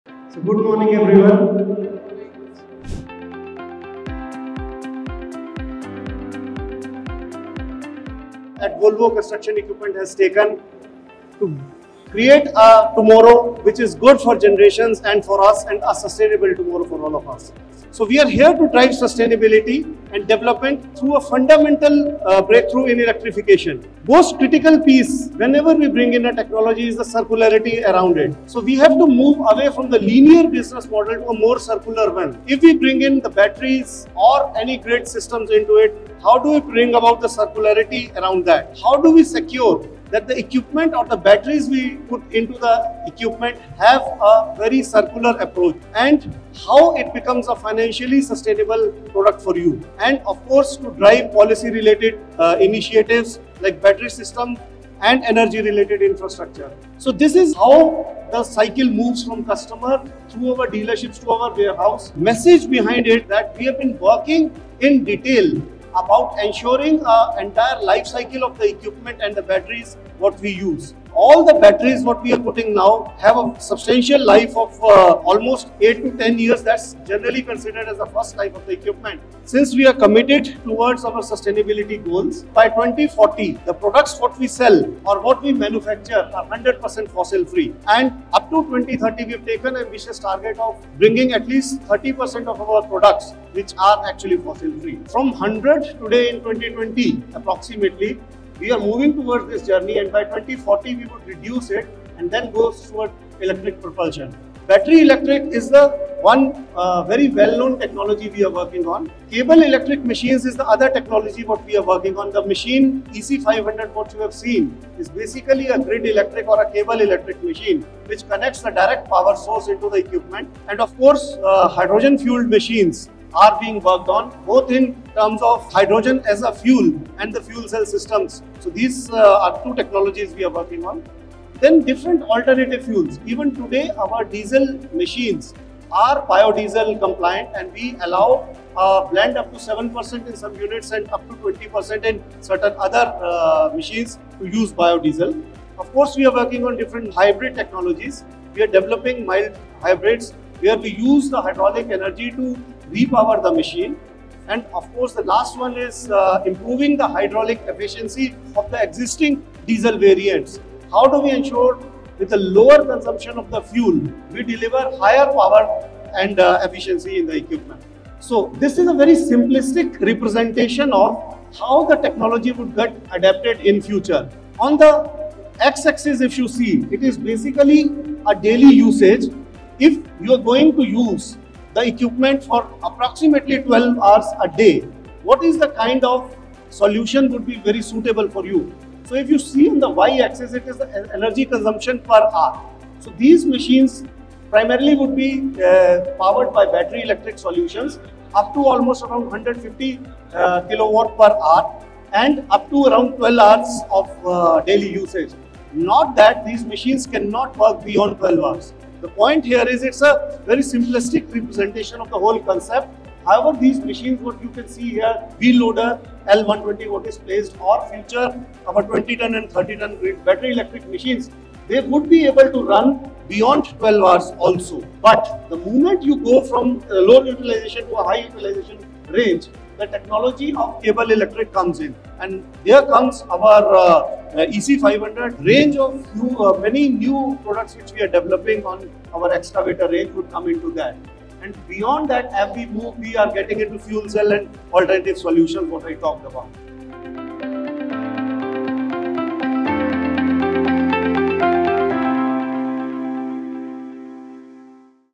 Electromobility in Construction with Surat Mehta | Volvo CE India's Vision for a Sustainable Future
Join Surat Mehta, Head of Productivity & Retail Development, Volvo CE, India to explore how electromobility is transforming the construction sector and how you can become a part of the transition!